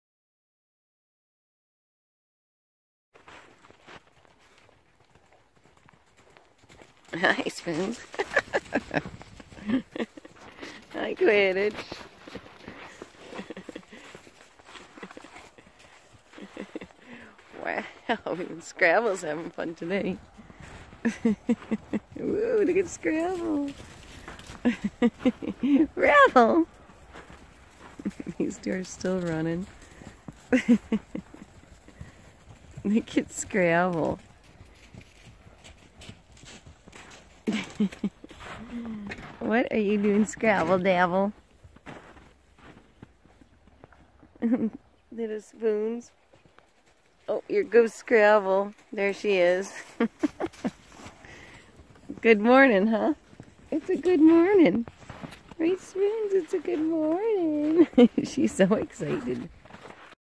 Happy horses playing in snow
A winter day that was actually a little warmer so the horses were very feisty.  They were happy to see their Mom and they breakfast.